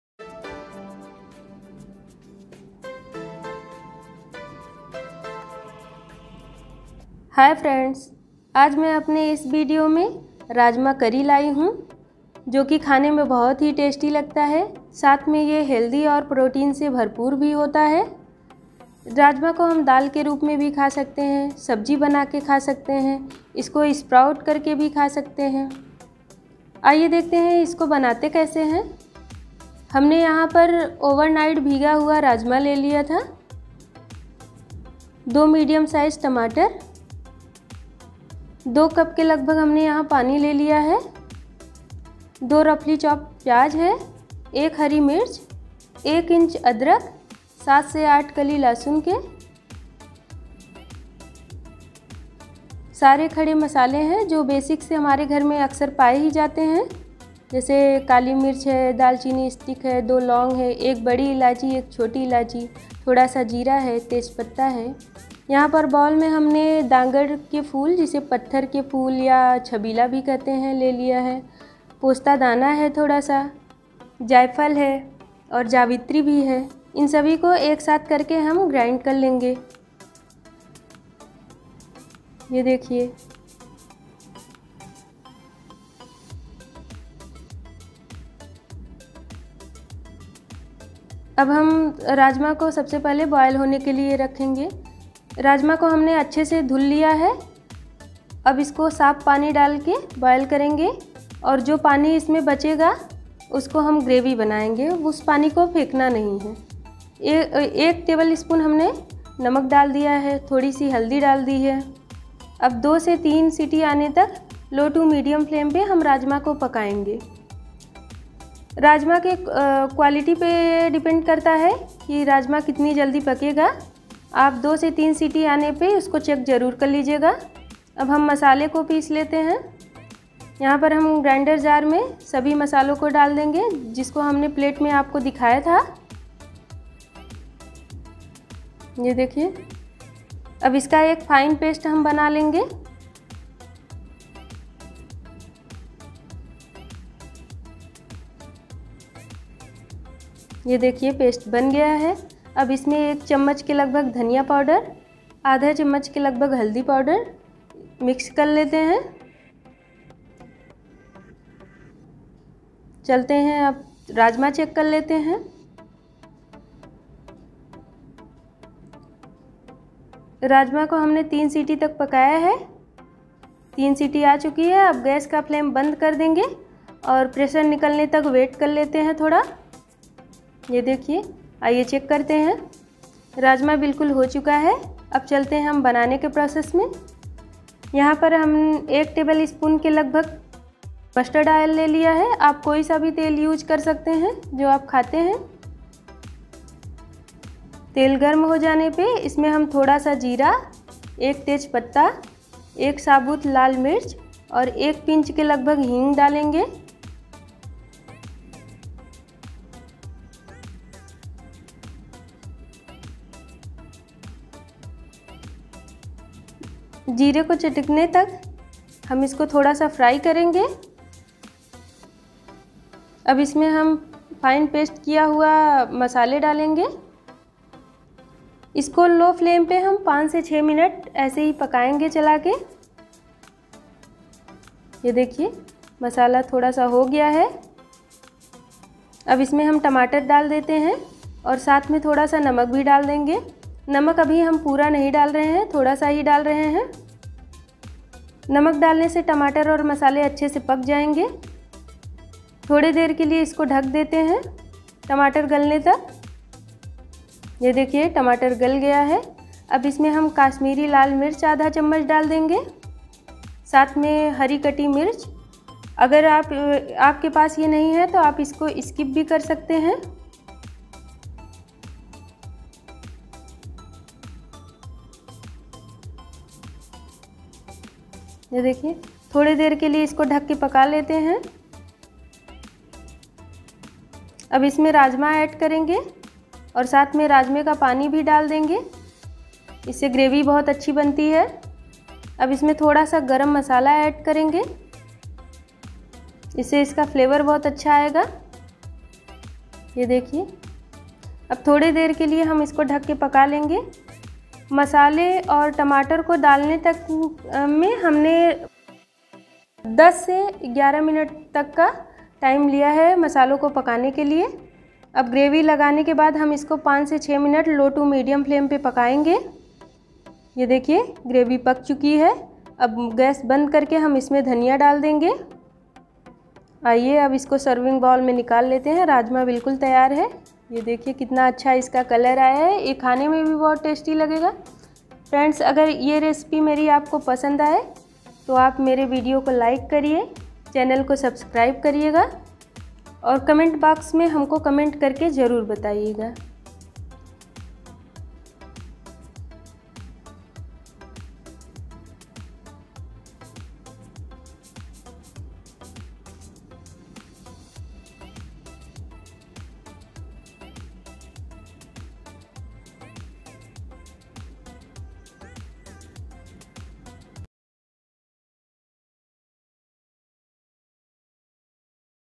Rajma Masala Recipe | पंजाबी स्टाइल राजमा मसाला | foody Tips | Rajma Masala Curry
how to cook Rajma ? rajma kaise banaya jata hai, Rajma recipe,